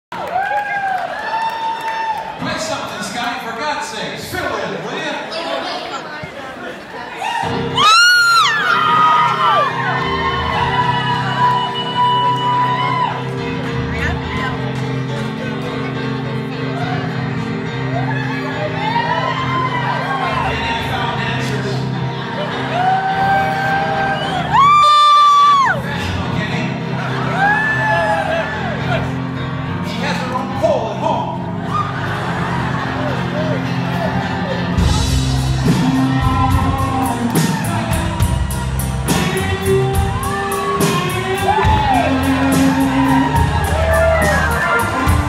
Chris Isaak Walkabout Phoenix 7/20/18
Chris Comes out into the audience